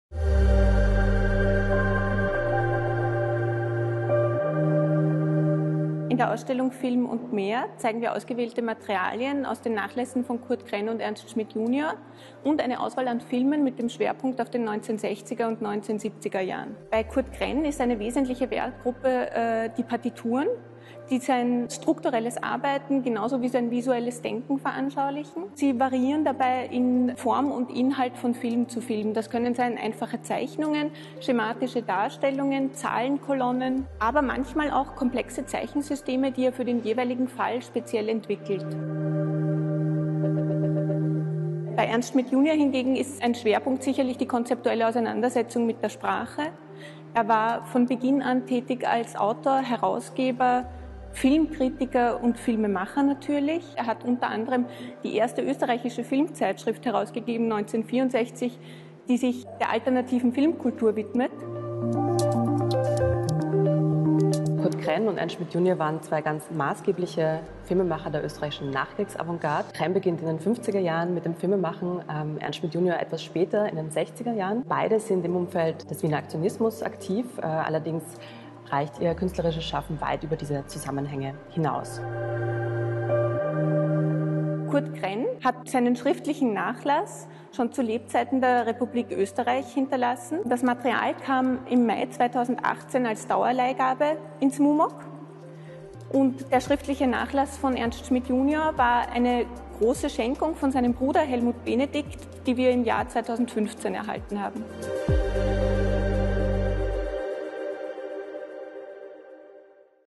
Das mumok zeigt den Nachlass von Kurt Kren und Ernst Schmidt jr. in einer neuen Ausstellung
Aus der Region Kultur und Information